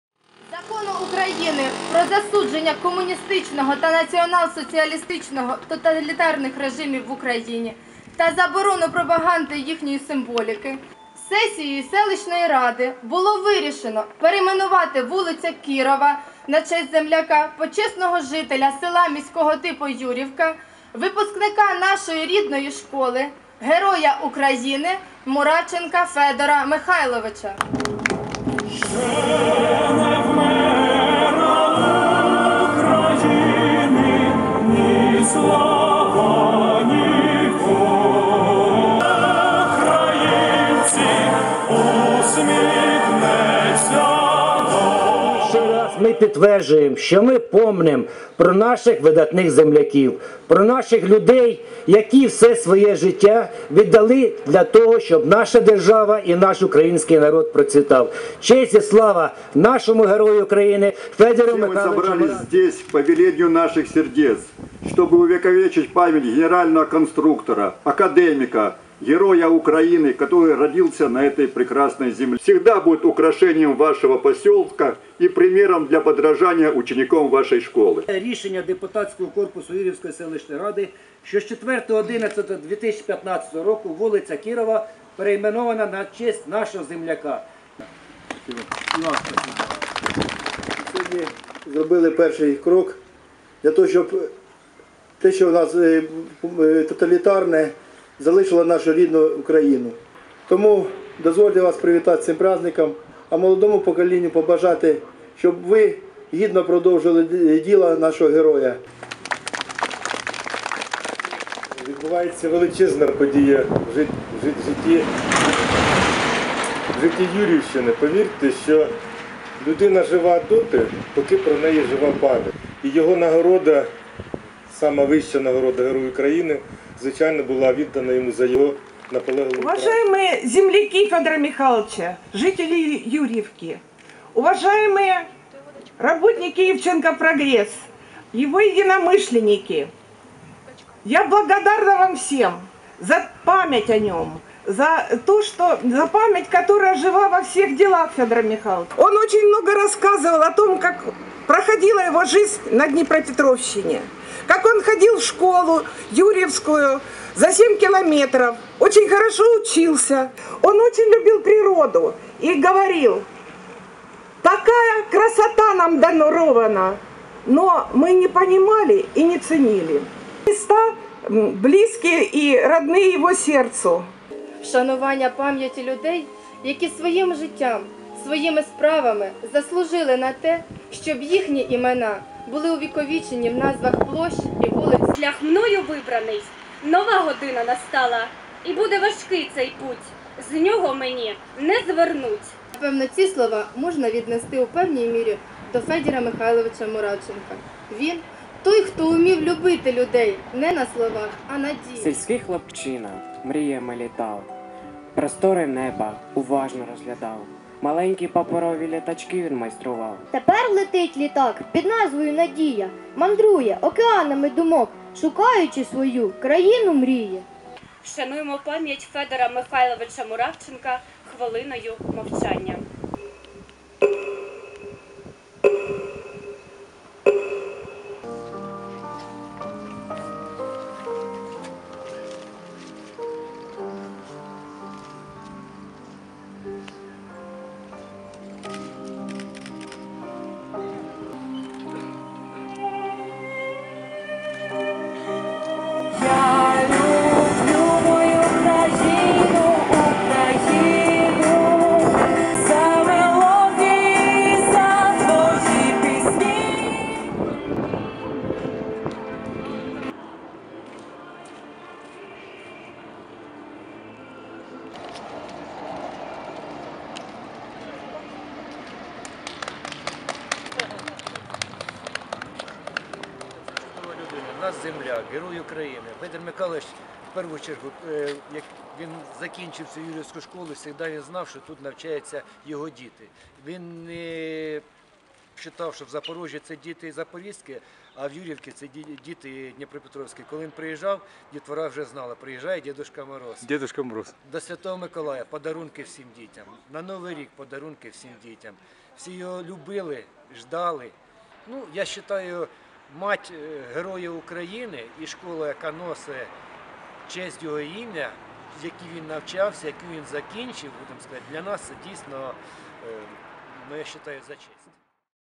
На Днепропетровщине появилась улица Героя Украины Федора Муравченко
Поселок Юрьевка Днепропетровской области. 18 декабря 2015 года. Торжественное открытие улицы с новым названием - имени Героя Украины Федора Муравченко.

Ф. Муравченко выпускник поселковой школы, известный украинский ученый, создатель авиационных газотурбинных двигателей и газотурбинных приводов, генеральный конструктор ЗМКБ “Прогресс”. Родился в 1929 году, юность провел на Юрьевшине, затем переехал в Запорожье, много лет проработал конструктором, имел почетные звания и награды. Ушел из жизни в 2010 году. Подробнее